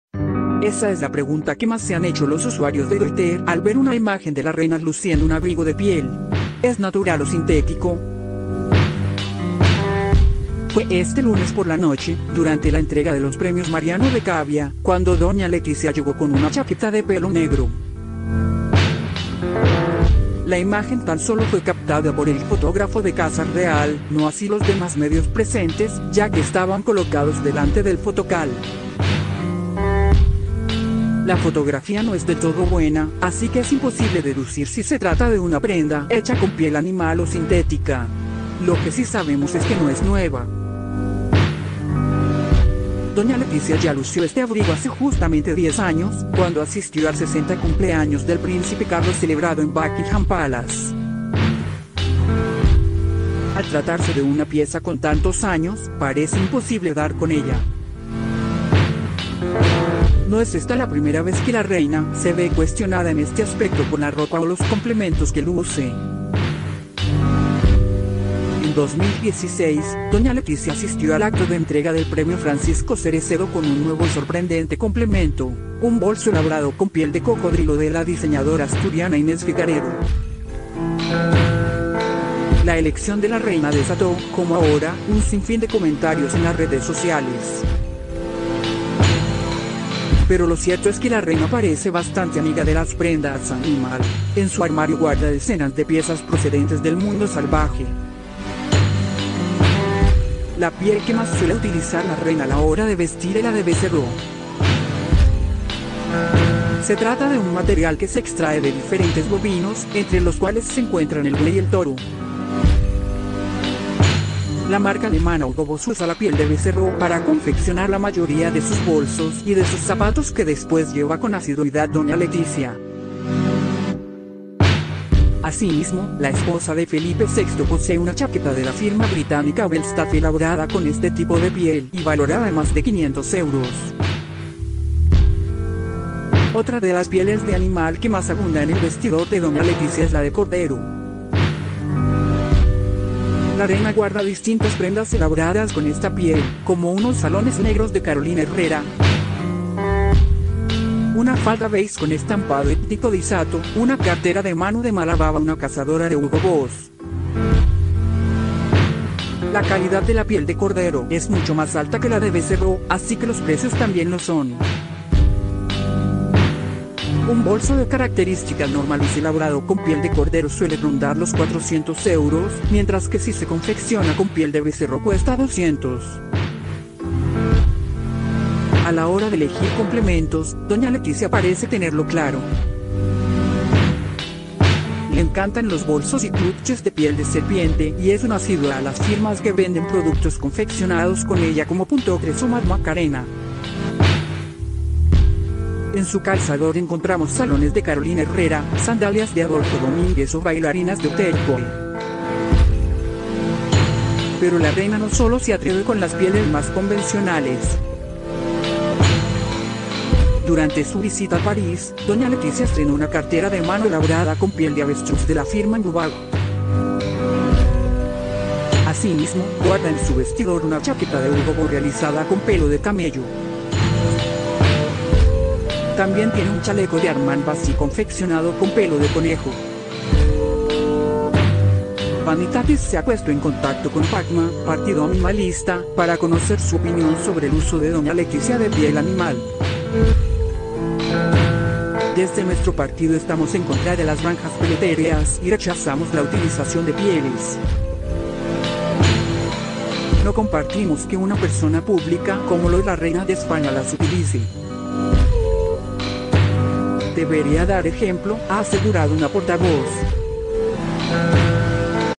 Nueva polémica por las pieles que habita la reina Letizia
La esposa de Felipe VI guarda en su armario desde una chaqueta de pelo de camello hasta un bolso de piel de avestruz